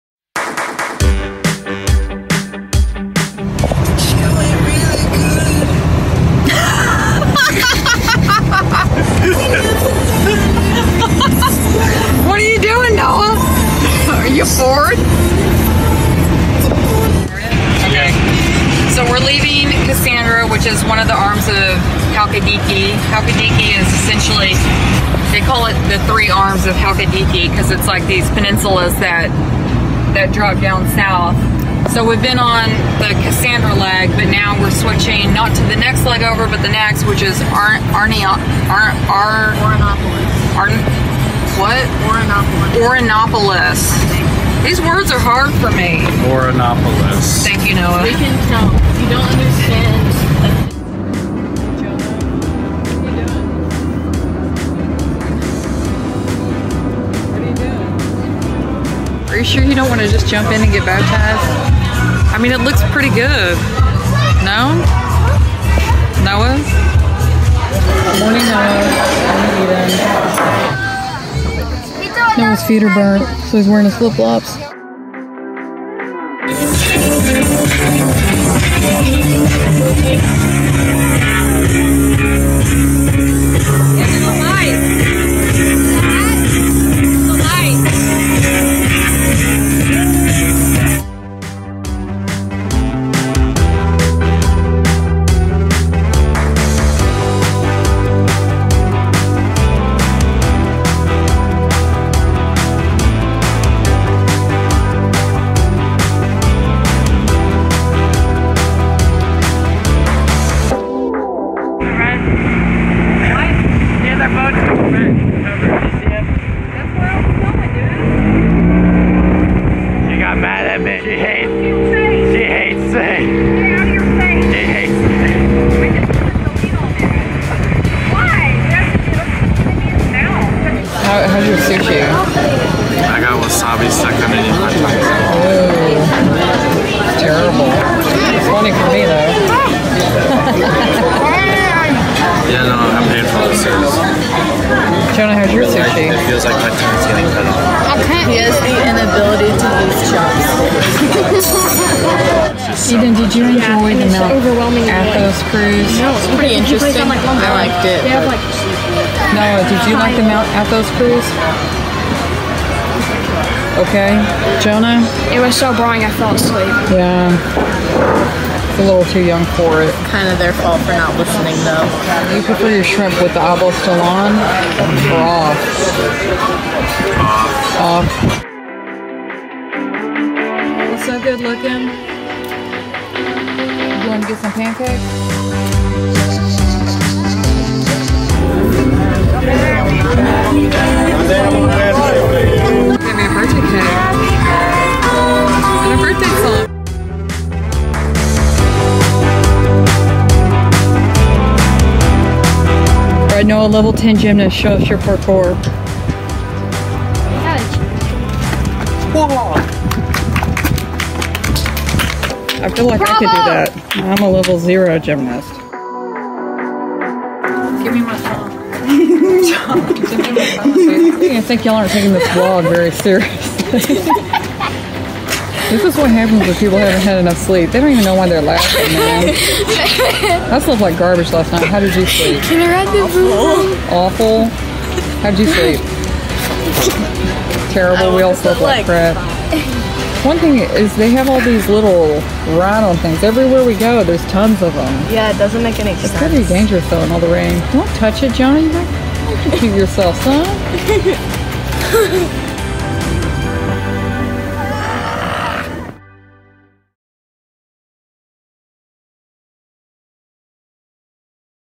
Vlog outtakes from Halkidiki, Greece
Stuff that didn’t make it into the other videos! Our outtakes while attempting to Vlog through Halkidiki, Greece